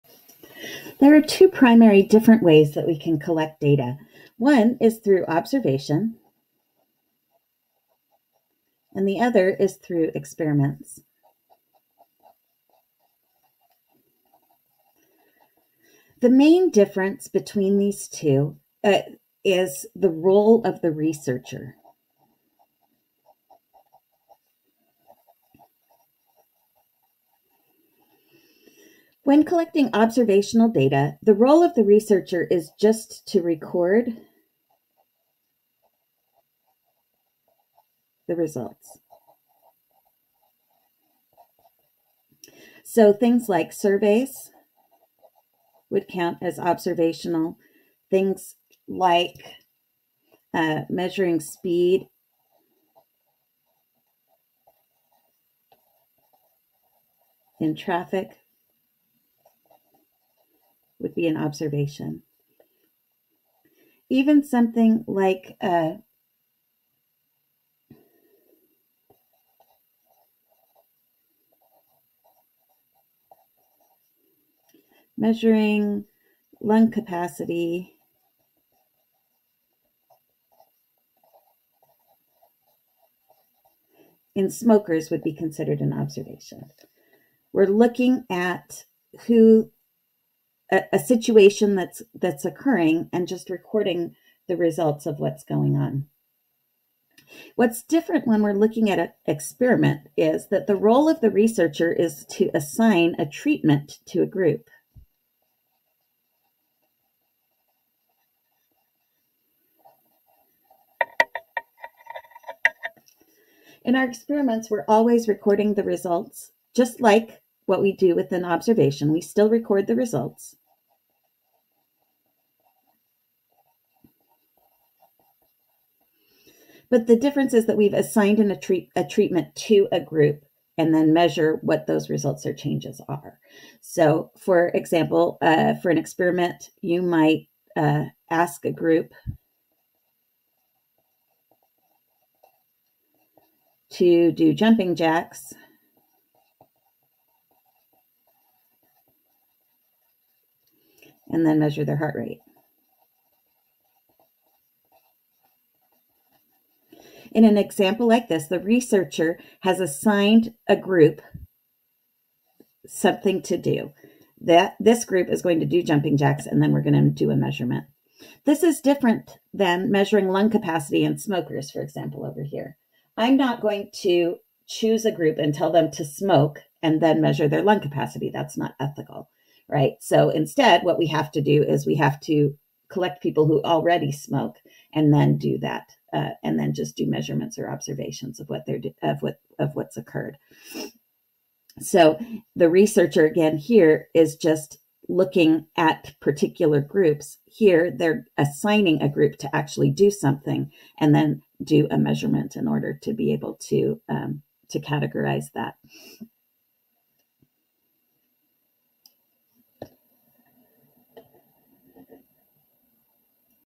Observations versus Experiments
Math in Society lesson introducing statistical procedures.  This video focuses on the differences between observations and experiments by looking at the researcher's role in data collection.